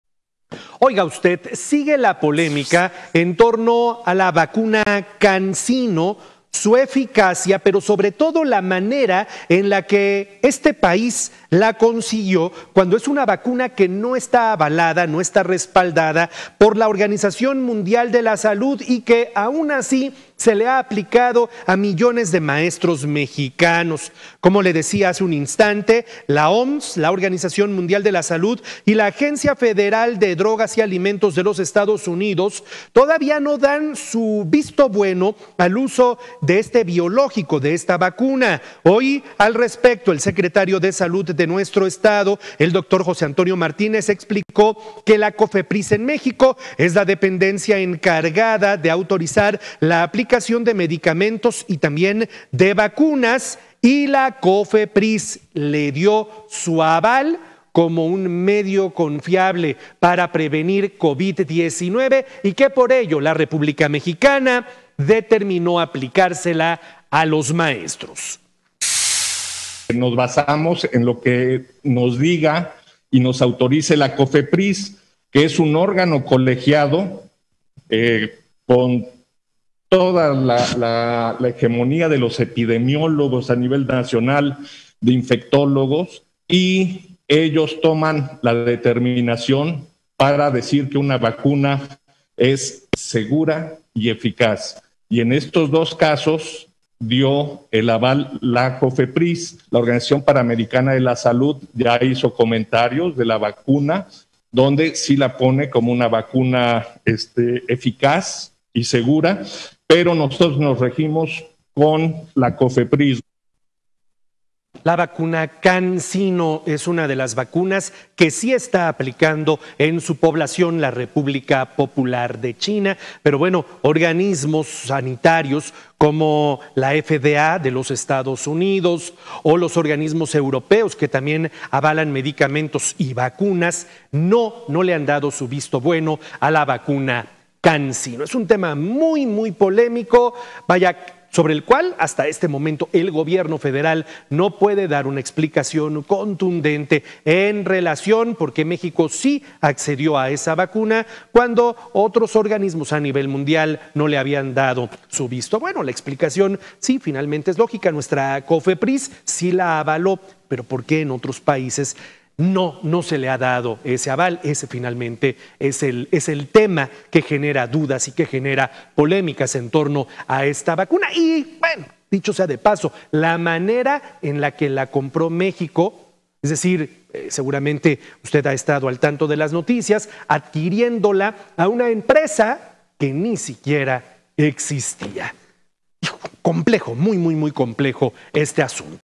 Polémica vacuna CANSINO || Noticias con Juan Carlos Valerio
Continúa la polémica con la vacuna Cansino que fue aplicada a los docentes mexicanos.